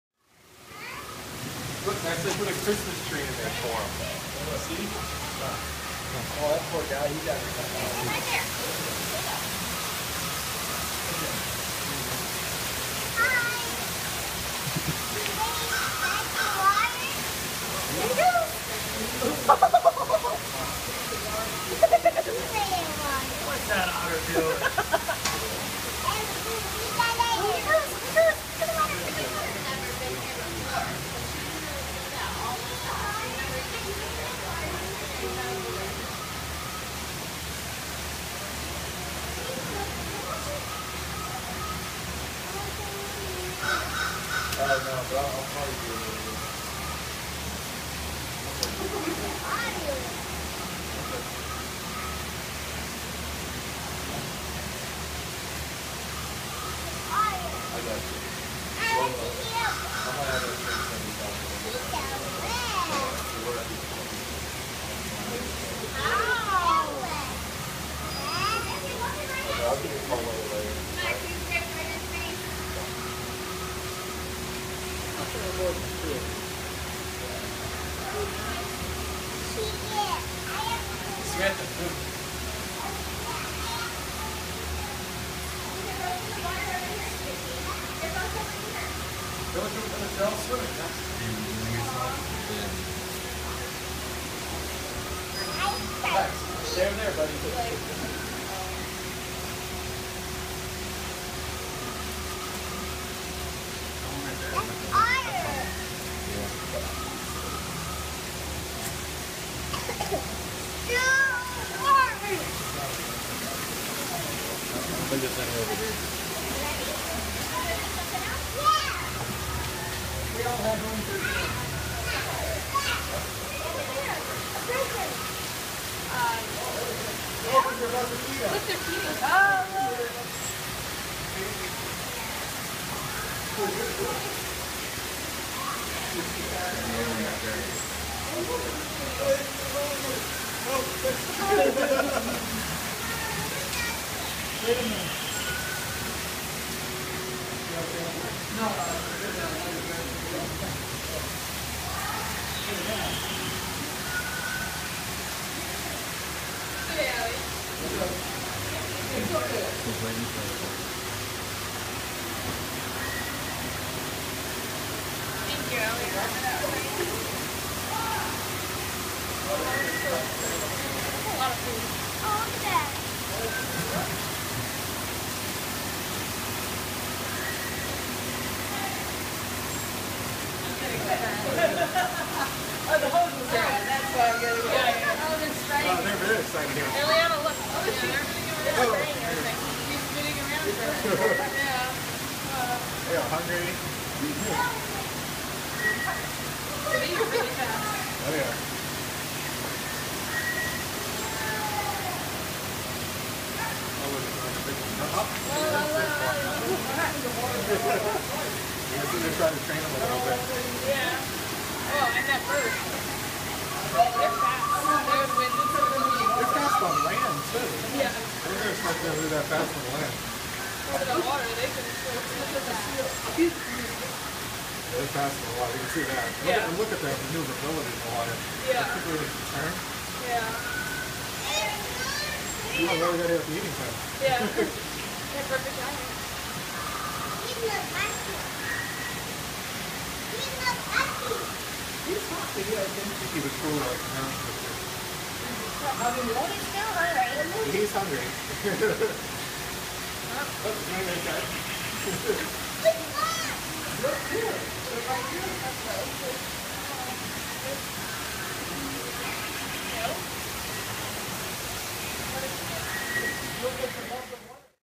Flip and Snacks check out some Otters
Went to the SC Aquarium where we got stabbed in the back watching a 4D movie about dino fish and visited the otters and spent over 100$ on otter plushies and got an otter penny. A good day.